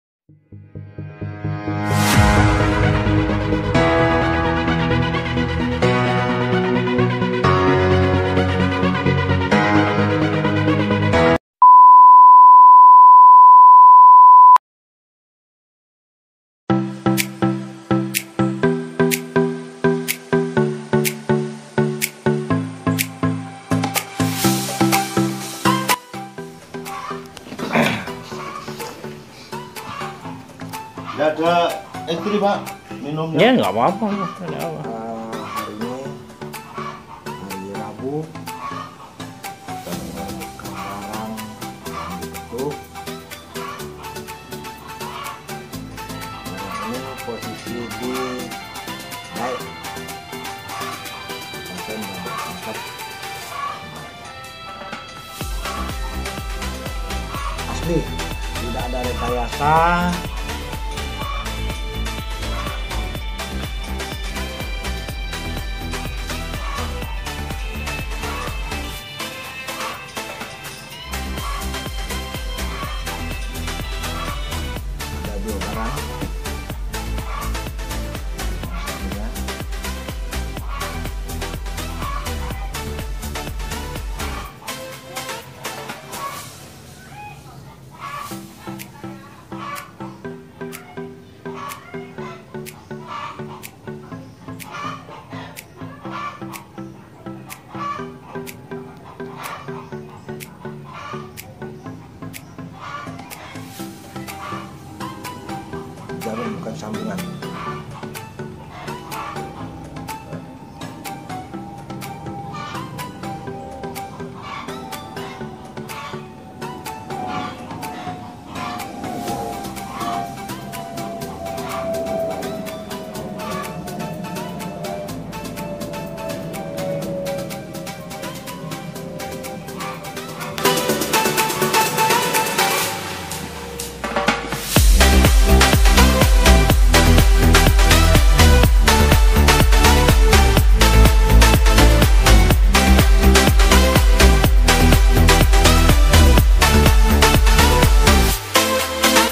BAMBU PETUK UKURAN JUMBOOO..!! ASLI / PALSU....??
Ok next video bambu petuk..
Yg ini kira2 asli / palsu ya.. ?

Yg tau jwbnnya silahkn komntr di bawah..